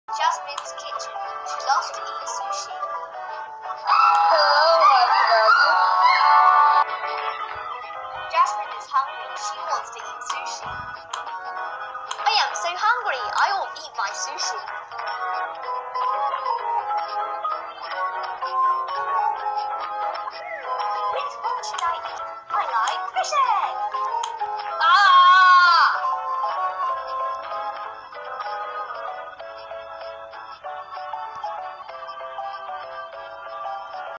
Anitales: "Sushi run" - gabrielgomes31
'This is Jasmine's kitchen. She loves to eat sushi.'. 'Sushi run' created by gabrielgomes31 using the Anitales app.